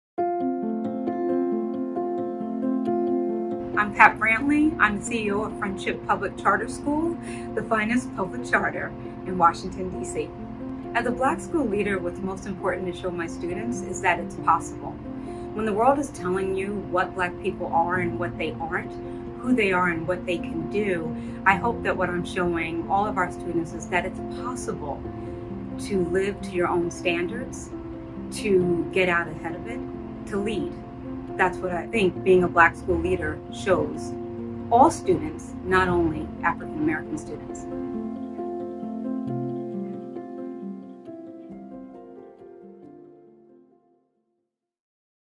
Making Black History in the Moment with Patricia Brantley, CEO of Friendship Public Charter School
Patricia Brantley, CEO of Friendship Public Charter School in Washington, D.C., on setting an example as a Black school leader.